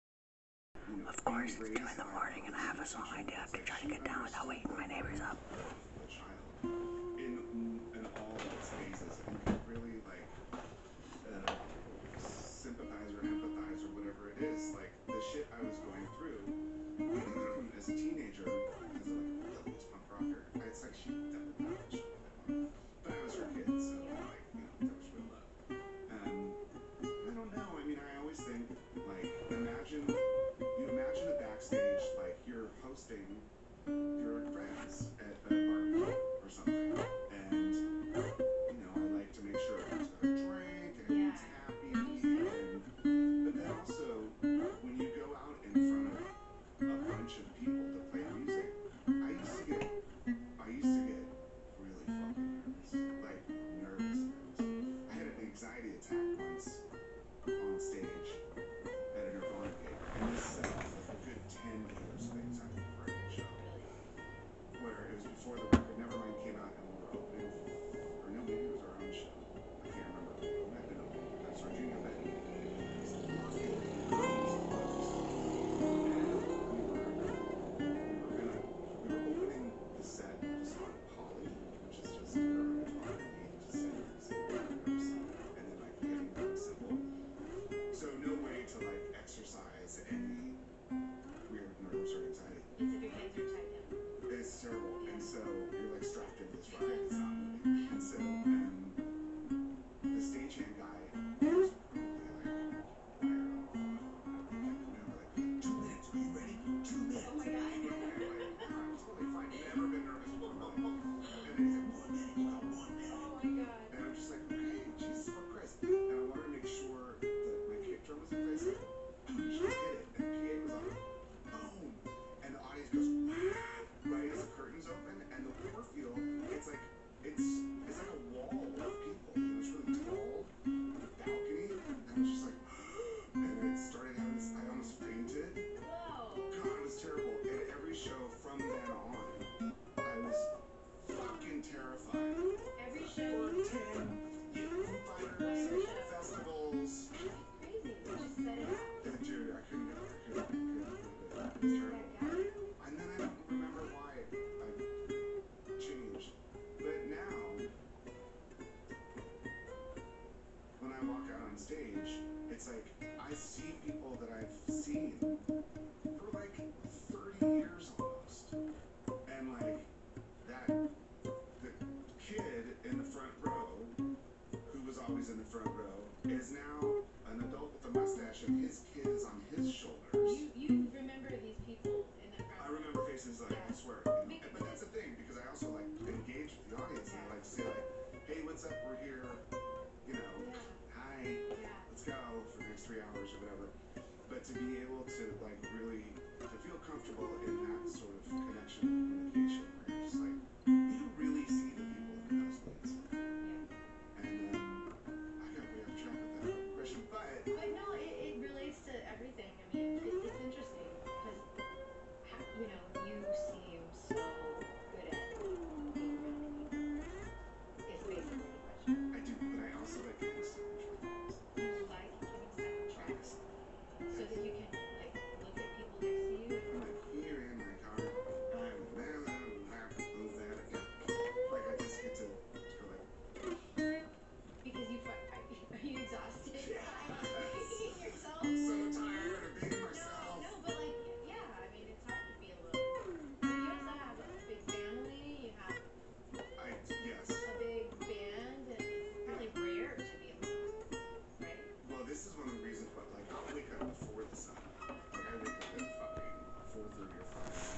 I can try all I want but I'm never able to shut off my brain enough to not write and play new things
live 2 am. Tuesday, er... Wednesday now...

I can try all I want, I can rip all the stings off my guitar... and I'll still NOT (NEVER!!!) be able to turn off my brAIN enough so I'm not writing.

damn Norah Jones is so talented.

and Dave Grohl is so me.
ha

hop in